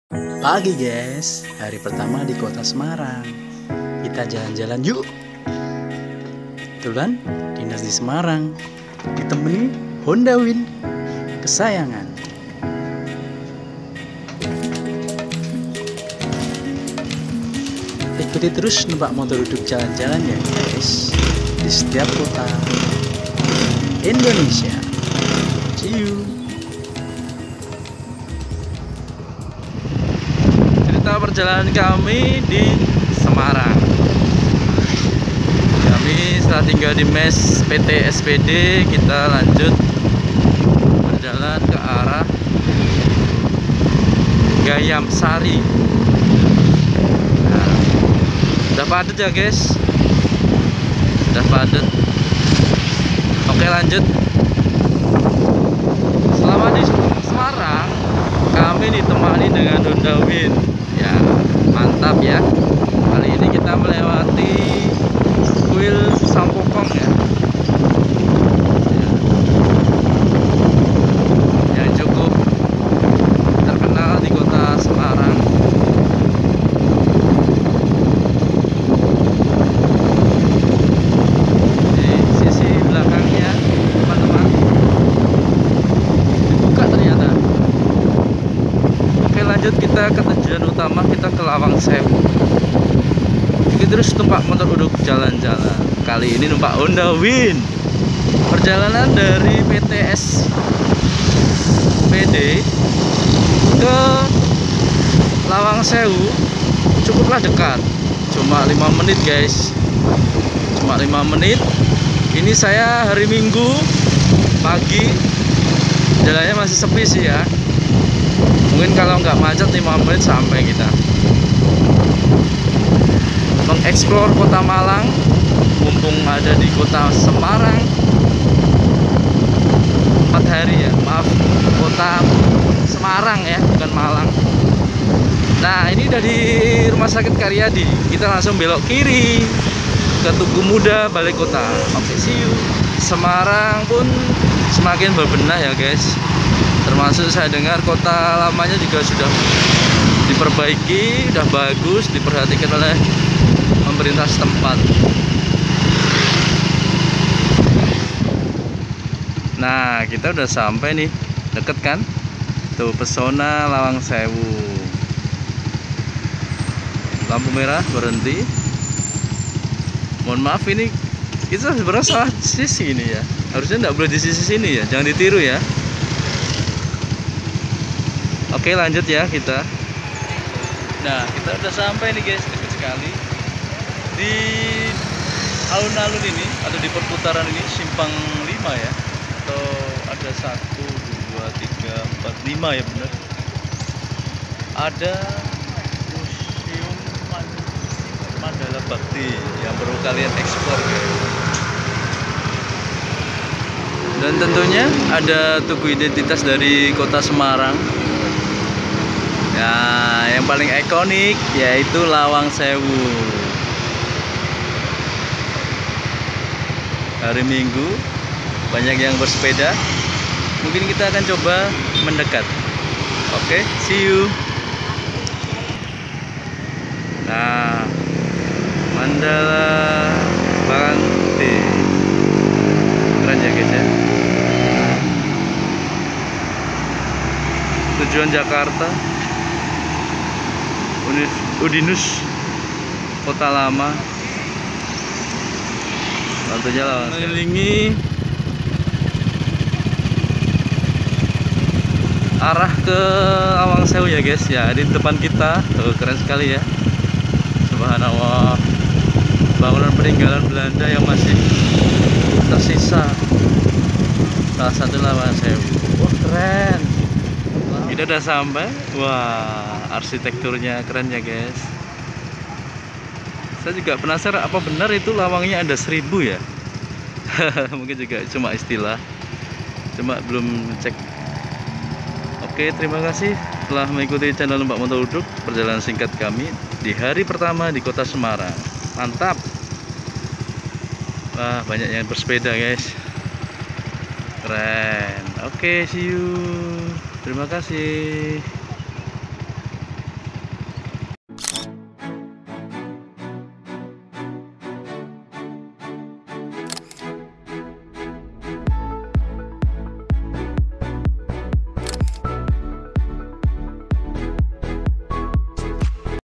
Jalan jalan ke Lawang Sewu Naik Honda Win
kali ini numpak motor udhug jalan jalan di kota semarang
melihat betapa indahnya peninggalan bangunan peninggalan belanda di kota semarang.

perjalanan singkat dari mess tempat menginap ke lawang sewu cukuplah dekat cuma lima menit.

sembari foto2 naik motor kesayangan...

sedap betul.. 😅🙏🏻